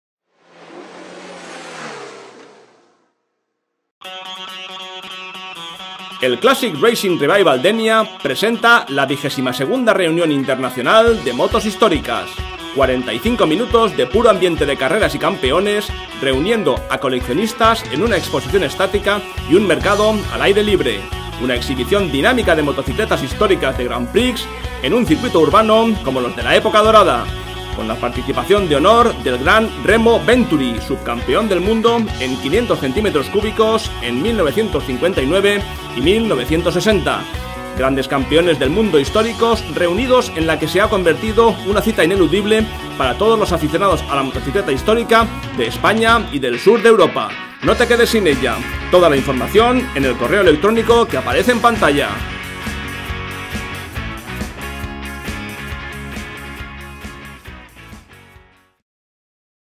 Racing Classic Revival Denia 2015 - Remo Venturi
Publicidad del Dvd
Producido por Carlos Bartual
Voz;  Javier Pérez , Radio Klara Vlencia